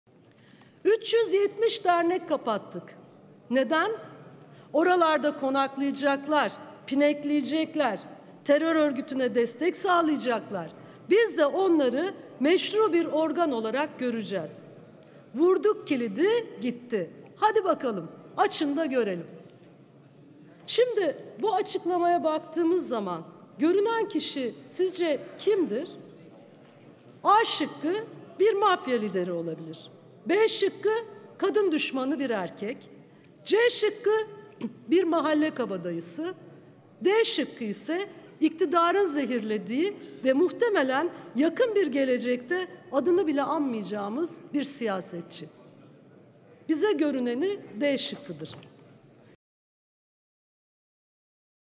Kerestecioğlu'dan şıklı Süleyman Soylu sorusu
Filiz Kerestecioğlu'ndan kolay bir soru: "370 dernek kapattık. Vurduk kilidi, gitti. Hadi bakalım, açın da görelim" diyen kimdir?